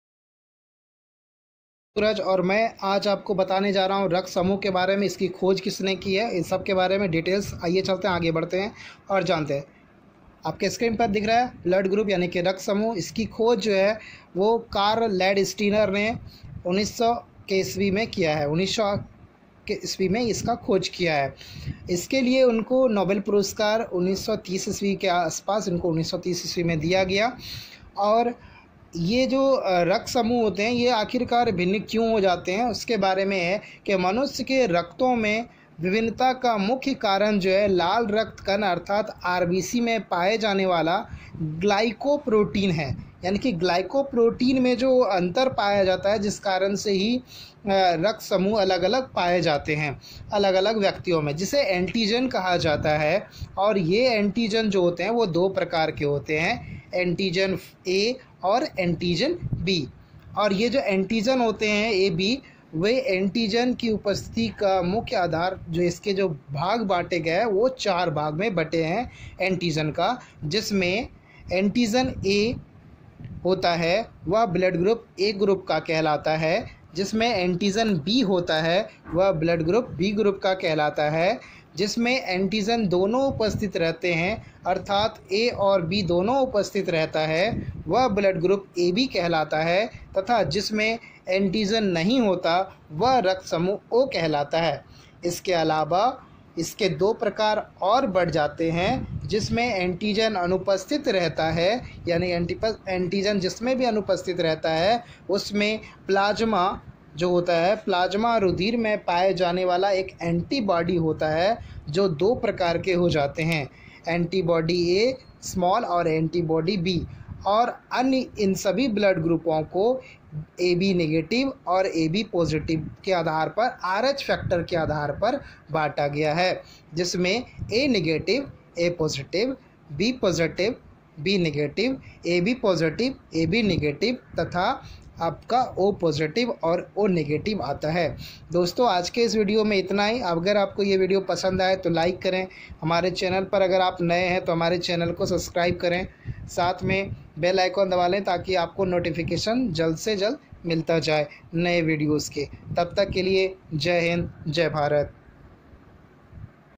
Blood group//रक्त समूह
वीडियो में जाने रक्त समूह के बारे में तथा उसके खोजकर्ता के बारे में में